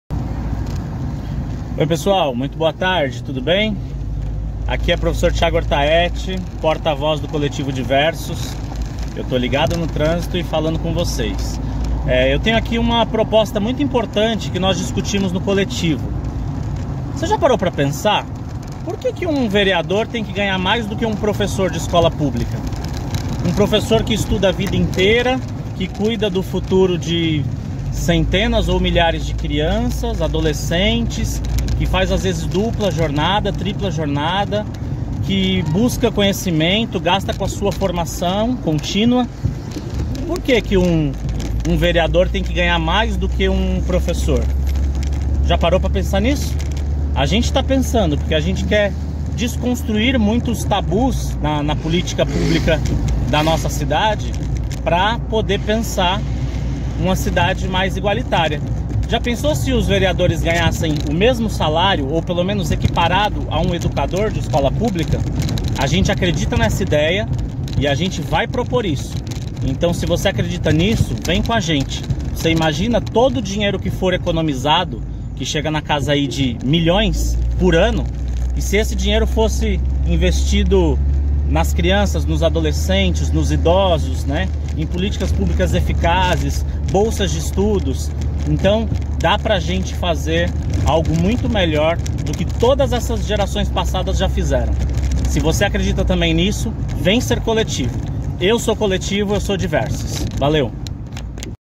POR QUE UM VEREADOR GANHA MAIS QUE UM PROFESSOR???
Eu tenho coragem para mexer nas estruturas, para ir contra o senso comum e as realidades estabelecidas, por isso mesmo sou pré-candidato a vereador, para propor o que nunca foi proposto na minha cidade, para provocar, refletir, fazer pensar... Você acha justo um vereador ganhar um salário absolutamente maior que um professor? Eu não acho! Um professor que estuda a vida inteira, que é responsável pela formação de todas as profissões, merece ser valorizado sempre. Que tal o legislativo de Guarulhos dar esse exemplo para o país inteiro??? Se eleitos, nós do @diversosmandatocoletivo vamos propor REDUÇÃO do salário dos vereadores.